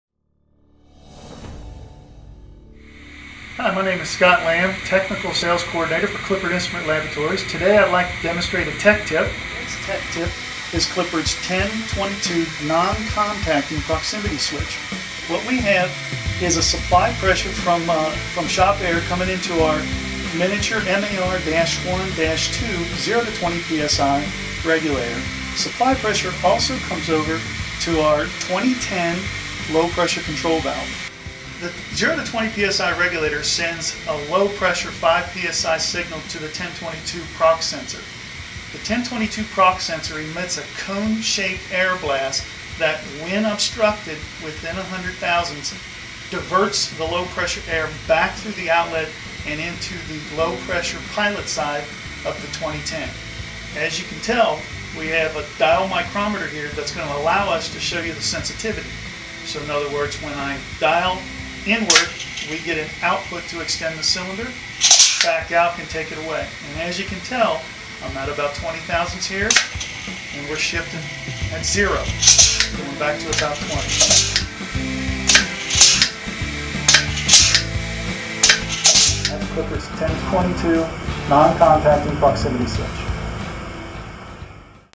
Tech Tip: 1022 Non-Contacting Proximity Sensor
Scott Lamb of Clippard Instrument Laboratory demonstrates Clippard's 1022 Non-Contacting Proximity Sensor.